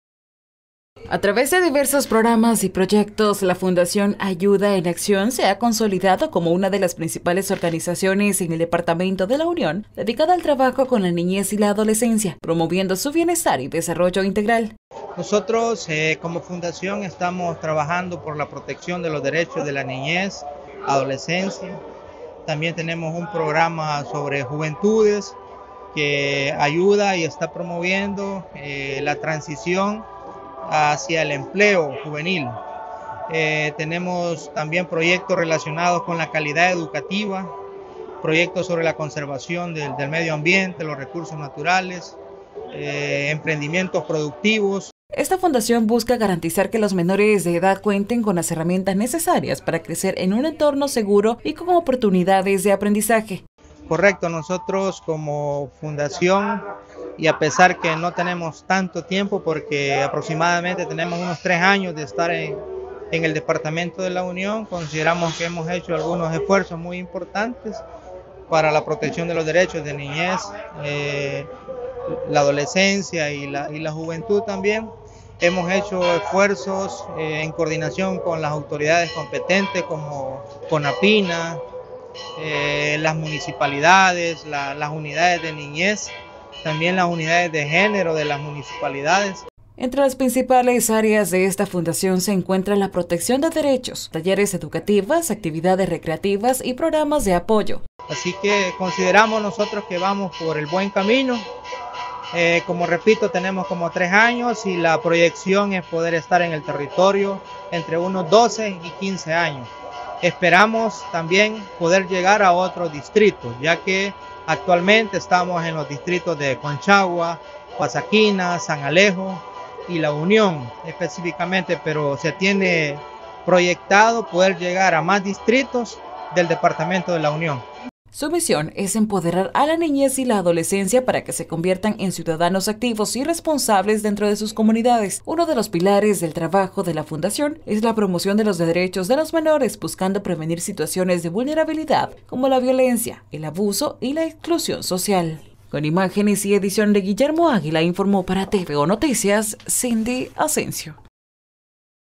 Fundación Acción Ciudadana se ha consolidado como una de las principales organizaciones en La Unión.
La Fundación Acción Ciudadana se ha consolidado como una de las principales organizaciones en el departamento de La Unión, dedicada al trabajo con la niñez y adolescencia, promoviendo su bienestar y desarrollo integral.